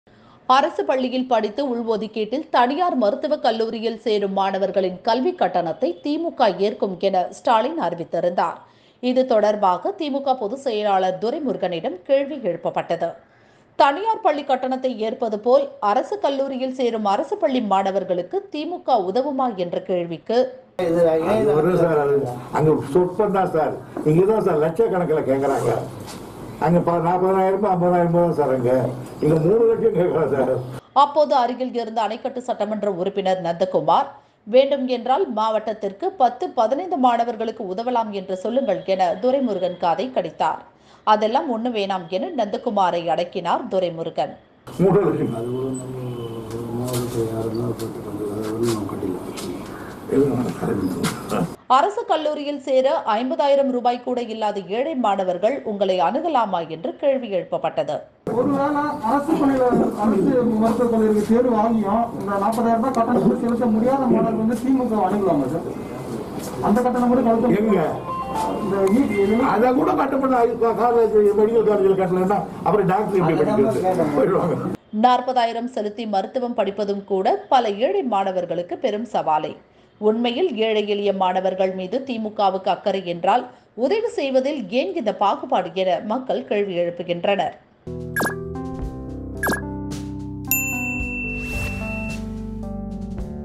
'கிளம்புங்க' என்கிறார் துரைமுருகன்
அரசுப் பள்ளியில் படித்து
உள் ஒதுக்கீட்டில் தனியார்
மருத்துவக் கல்லூரியில் சேரும்
மாணவர்களின் கல்விக் கட்டணத்தை
திமுக ஏற்கும் என ஸ்டாலின்
அறிவித்திருந்தார்.